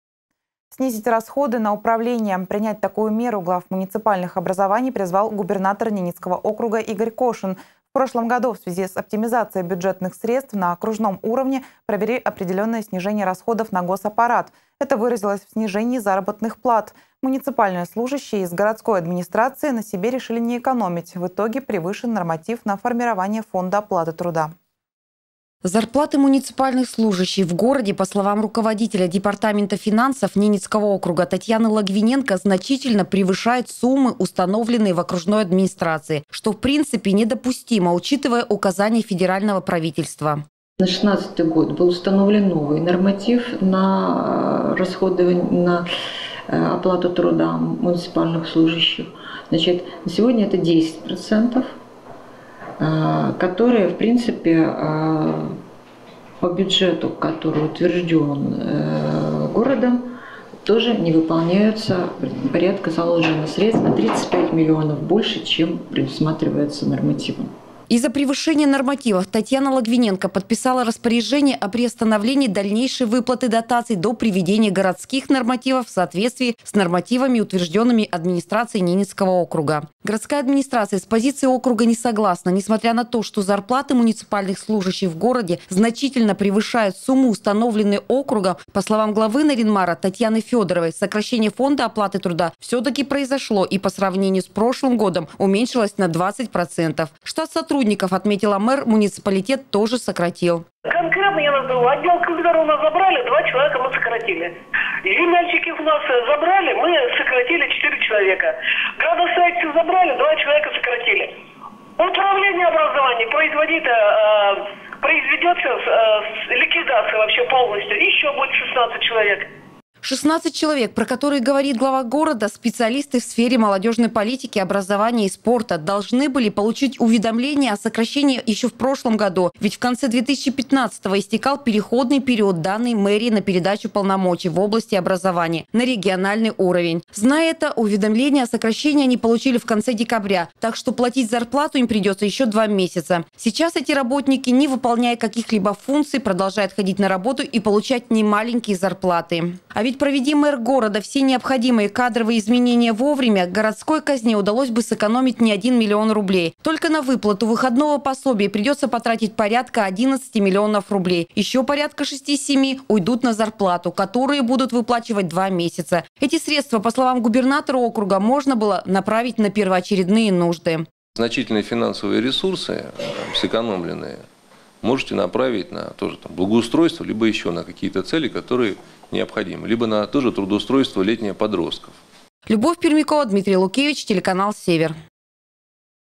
Фонд оплаты труда в администрации Нарьян-Мара по-прежнему превышен
Снизить расходы на управление. Принять такую меру глав муниципальных образований призвал губернатор Ненецкого округа Игорь Кошин. В прошлом году в связи с оптимизацией бюджетных средств на окружном уровне провели определенное снижение расходов на госаппарат. Это выразилось в снижении заработных плат. Муниципальные служащие из городской администрации на себе решили не экономить,  в итоге — превышен норматив на формирование фонда оплаты труда.